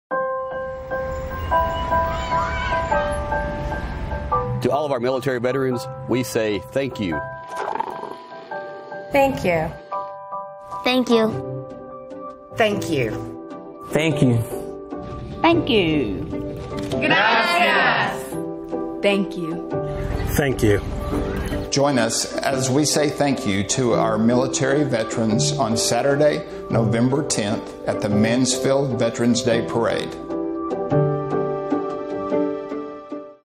2018 Veterans Day Parade "Thank You"
Join the City of Mansfield and MISD as we say thank you to the men and women of the military with the 2nd Annual Veterans Day Parade & Salute at 10 a.m. on Saturday, Nov. 10, followed immediately by a patriotic concert at R. L. Anderson Stadium.